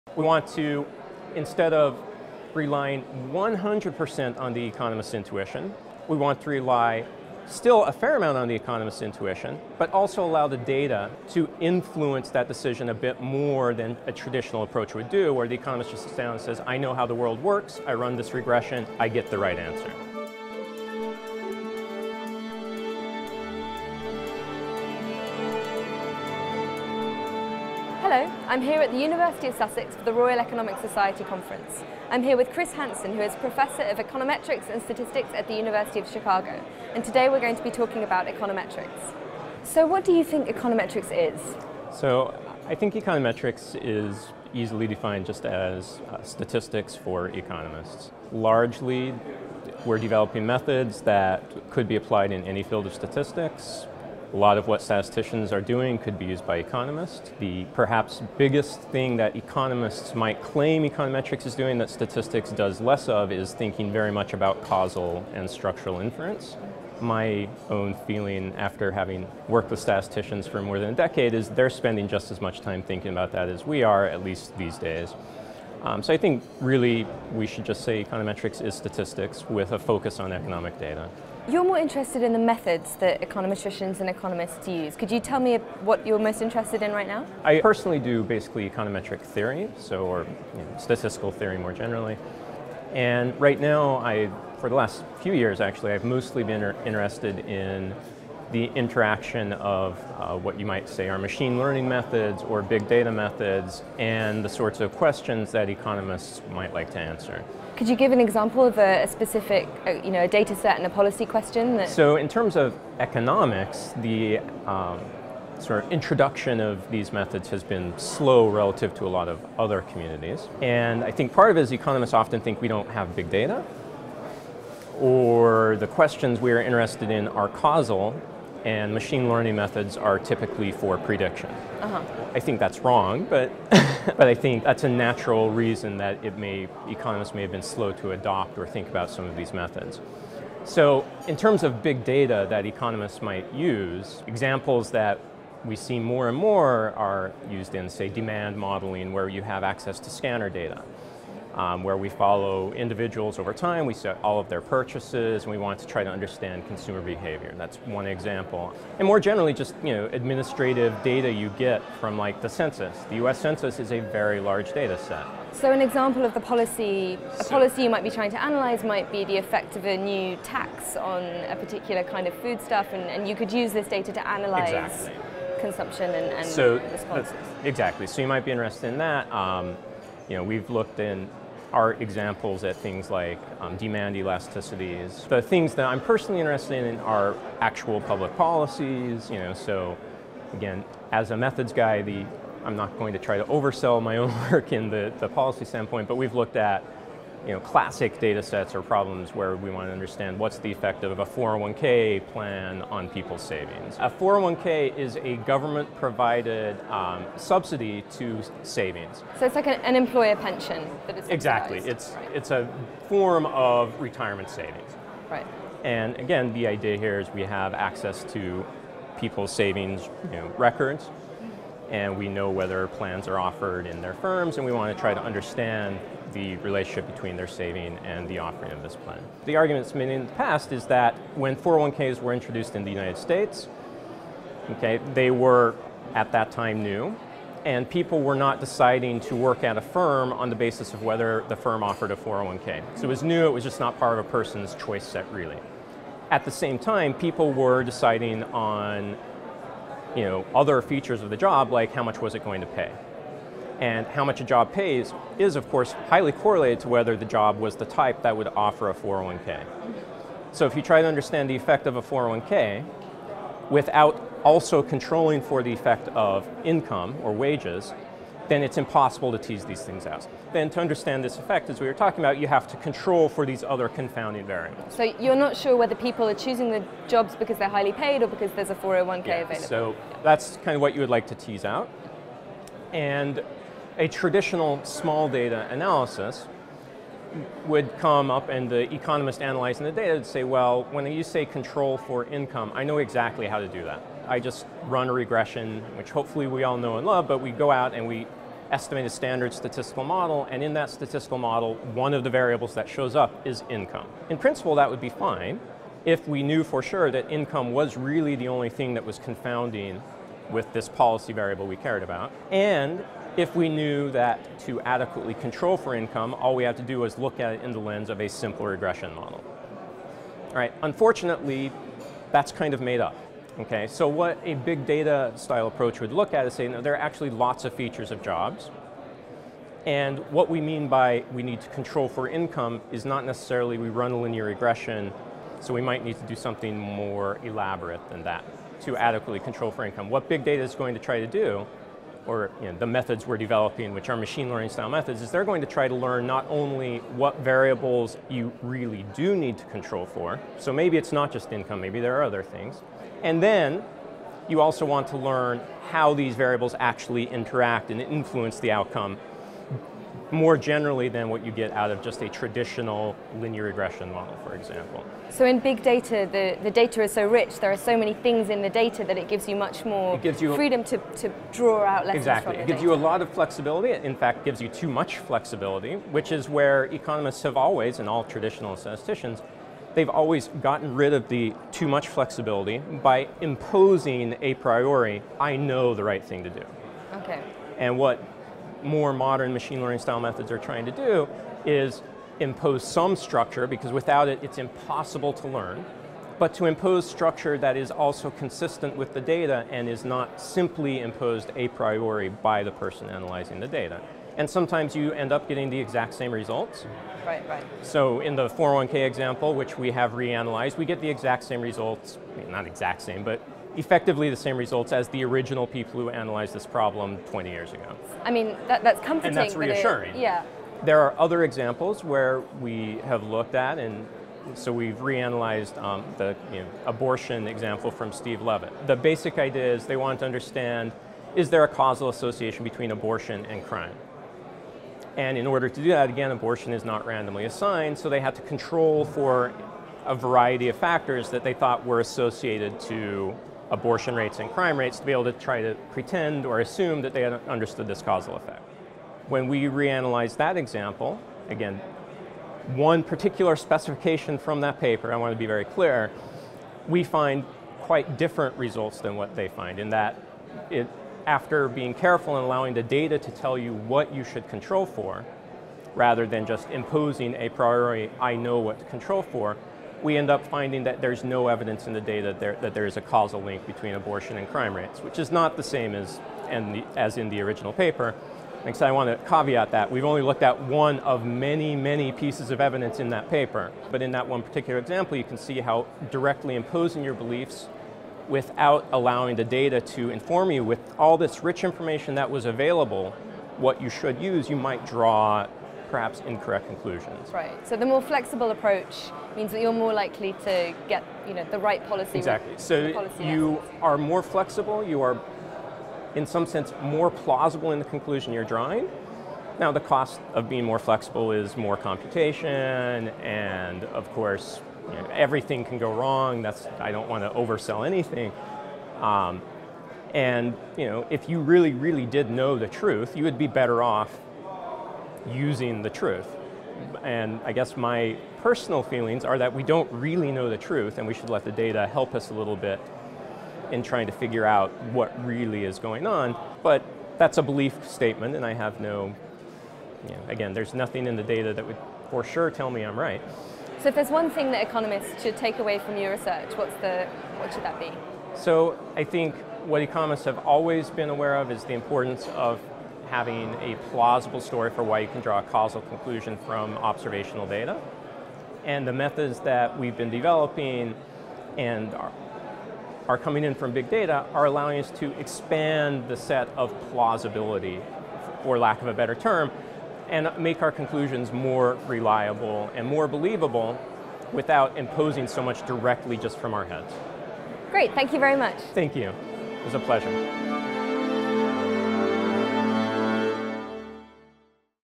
Using Big Data: An Interview with Christian Hansen - RES 2016
Christian Hansen (University of Chicago Booth School of Economics) is interviewed by Soumaya Keynes (The Economist) about econometrics and his work on the best methods to use to understand Big Data.

Produced at the Royal Economic Society's annual conference at the University of Sussex by Econ Films.